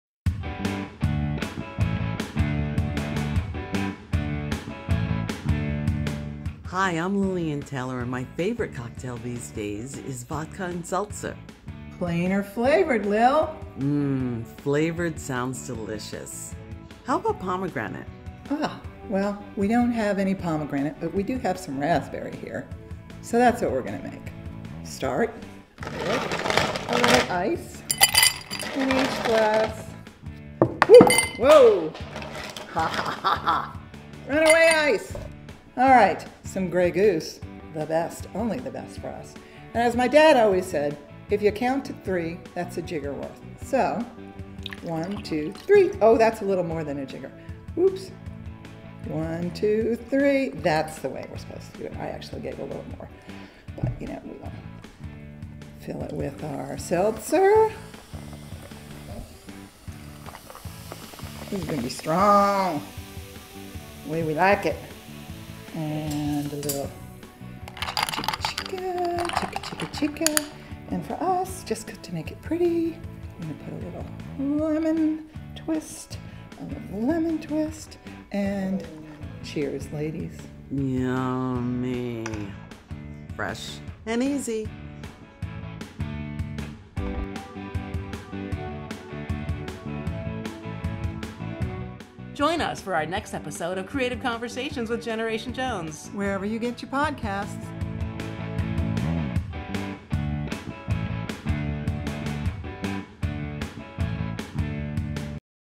S2E3 Cocktail Hour - Mixing a Vodka and Seltzer - with Lillian Taylor
Humorist and blogger Lillian Taylor tells us how to make her simple and delightful favorite cocktail, a vodka and seltzer.

Check her out at her website and on her social media and get ready to laugh at her fabulous funny memes. And pick up a mug from her snarky mug collection for your morning cup of caffeine!